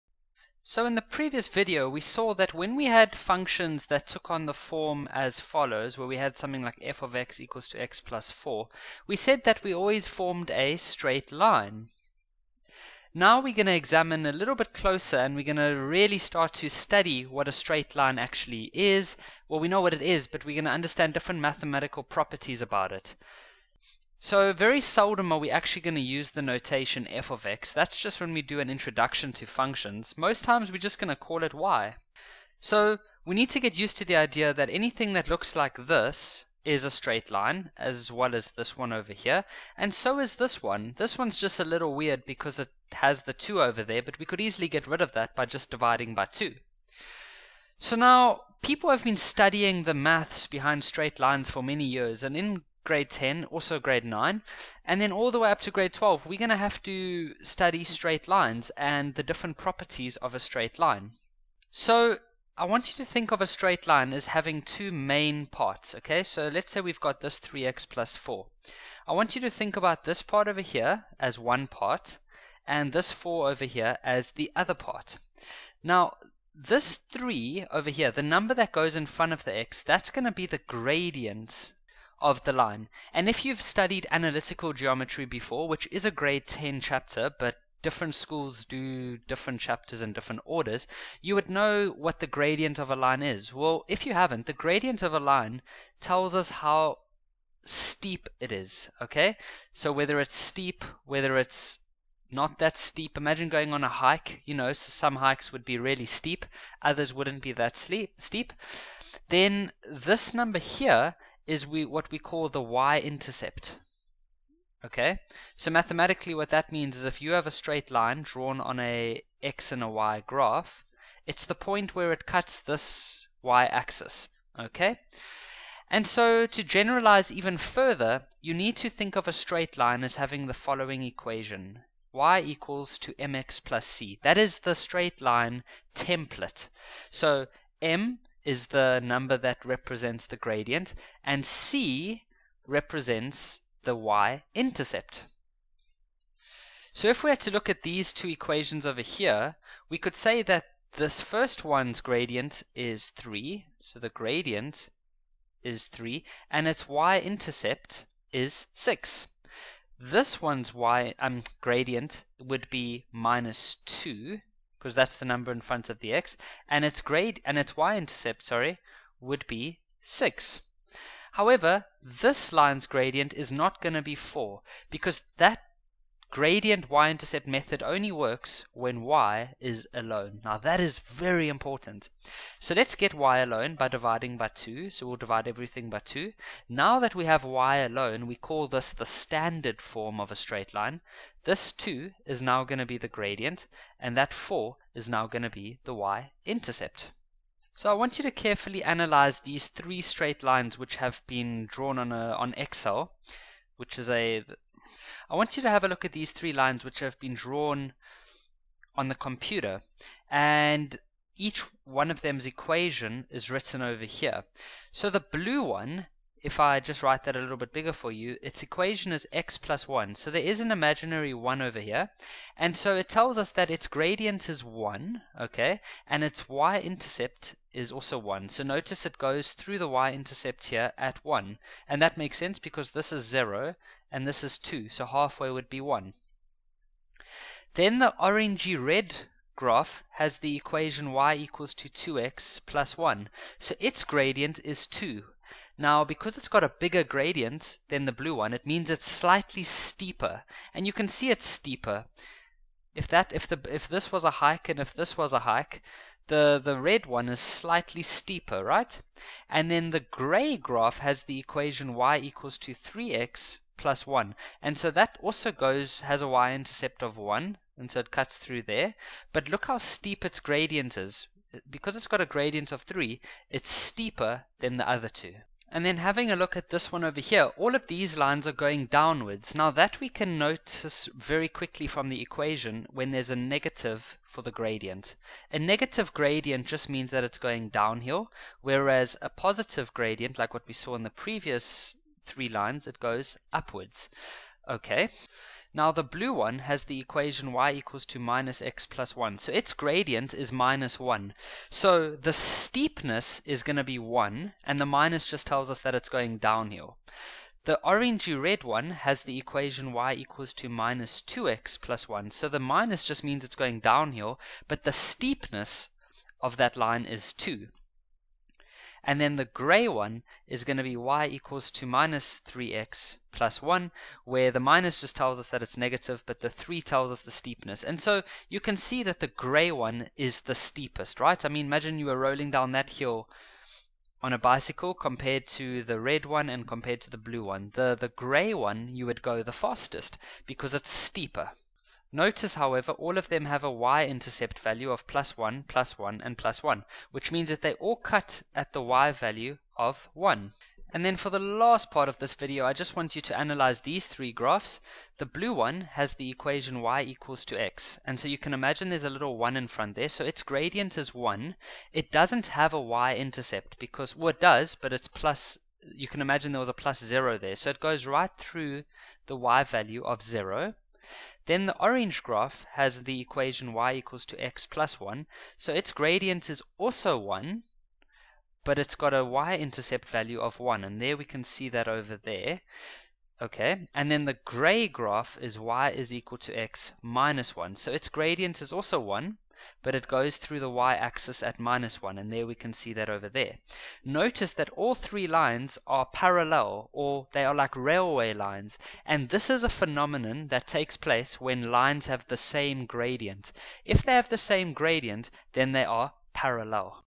Straight Line Grade 10: y = mx+c
In this maths lesson we learn about y = mx + c in grade 10 straight lines.